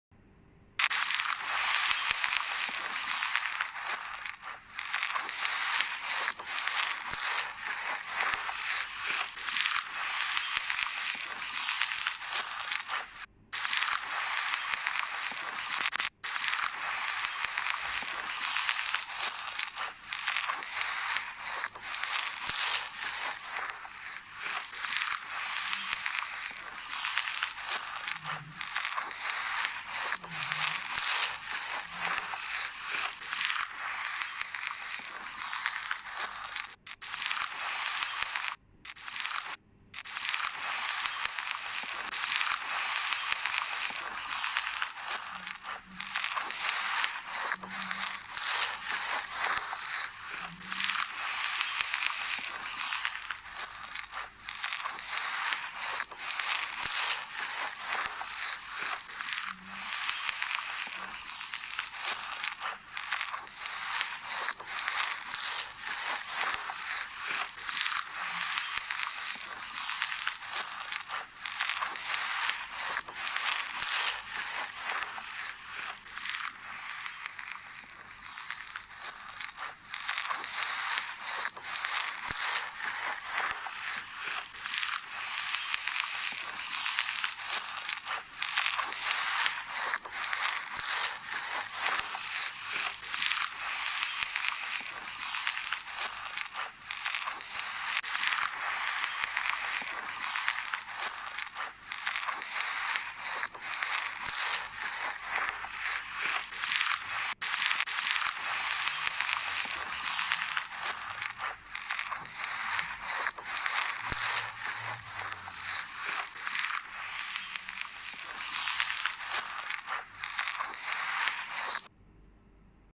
😉(Houny Crushing Sound(SLIME
A mixture of slime and honey gives you comfort 🤗🤗and calm